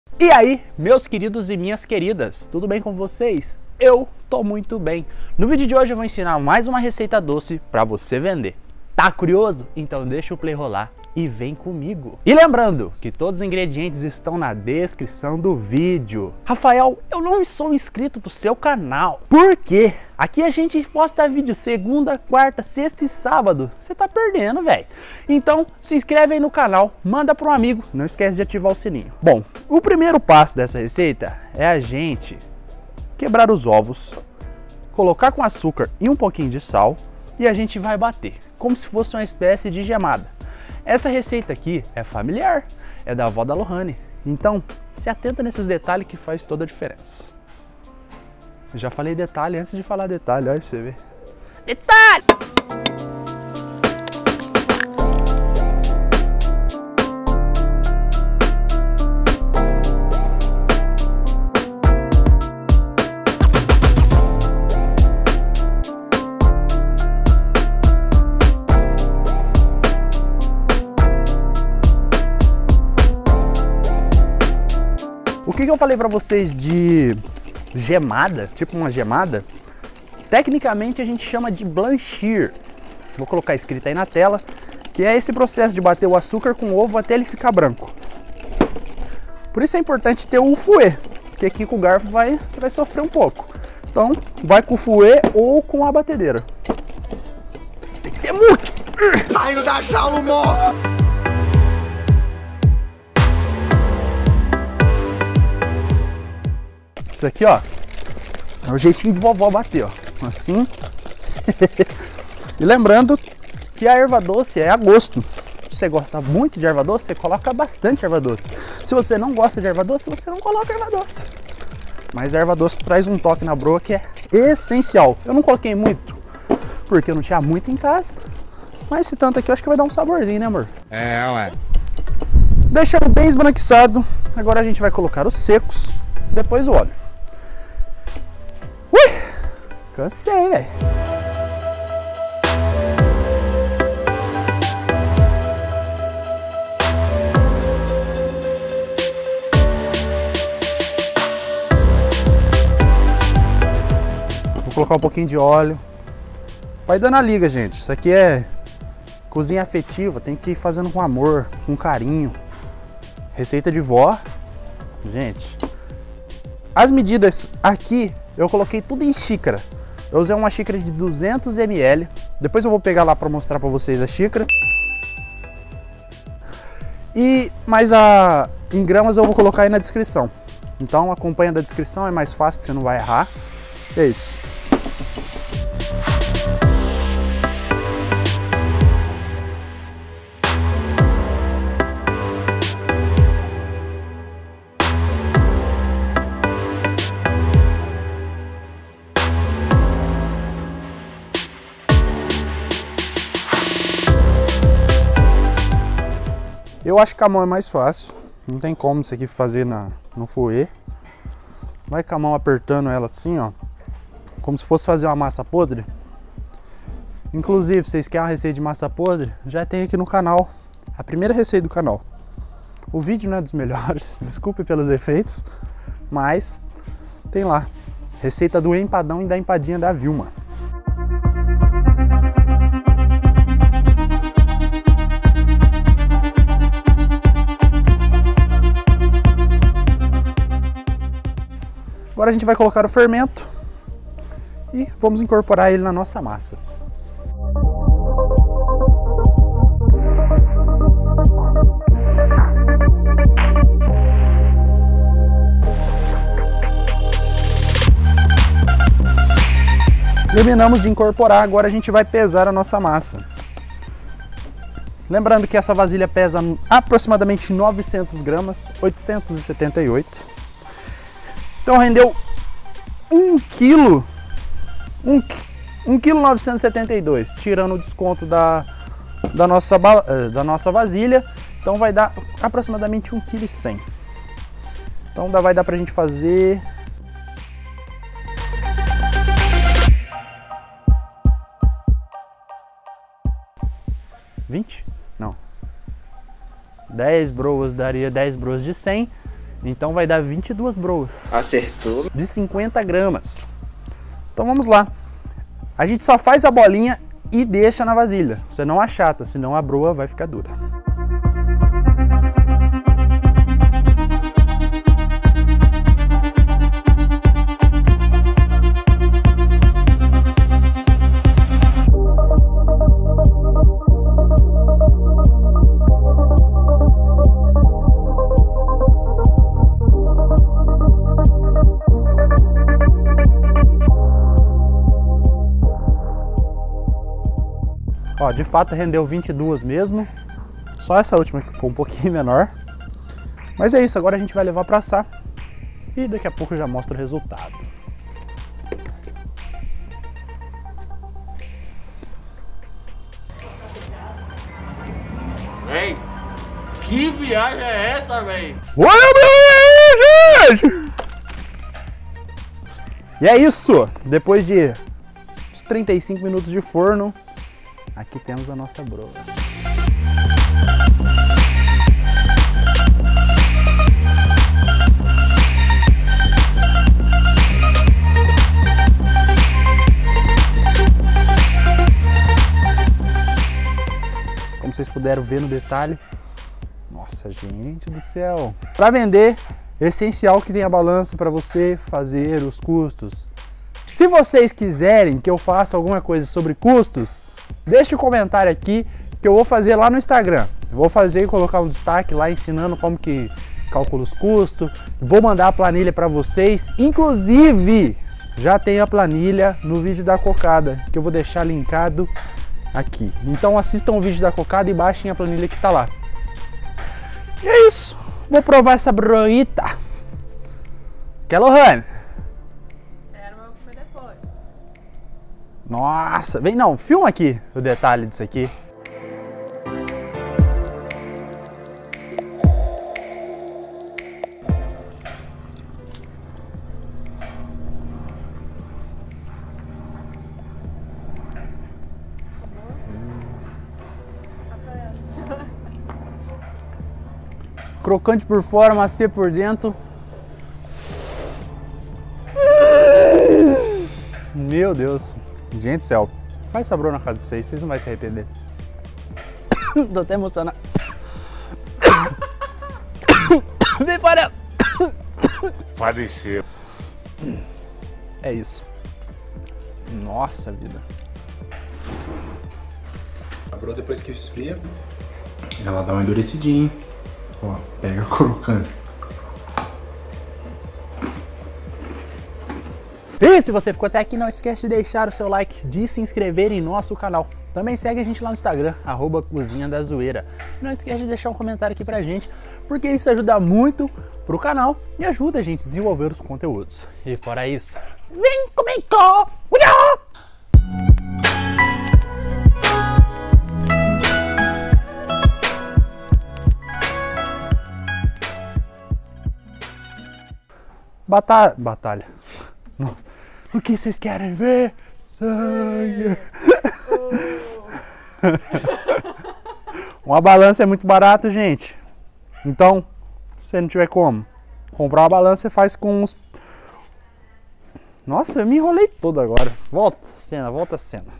FAÇA E VENDA: BROA DE FUBÁ SIMPLES E BARATA | RECEITA DOCE
Ingredientes:
450g de fubá (3 xícaras)
130g de farinha de trigo (1 xícara)
250g de açúcar (1 xícara)
200g de óleo
1 pitada de sal (2g)
10g de fermento químico (1 colher de sopa)
2 ovos inteiros
Este vídeo é para você que quer começar a vender algum tipo de alimento, então neste vídeo vou te ensinar como fazer uma deliciosa broa de fubá, esta receita de broa de fubá é uma receita passada de geração em geração na família da minha noiva, então não perca essa receita de broa de fubá que além de ser deliciosa, tem um custo muito baixo e é bem legal para iniciar as suas vendas ou incrementar ainda mais com as coisas que você faz! Se você fizer esta broa de fubá, não esquece de marcar a gente no Instagram (@cozinhadazueira)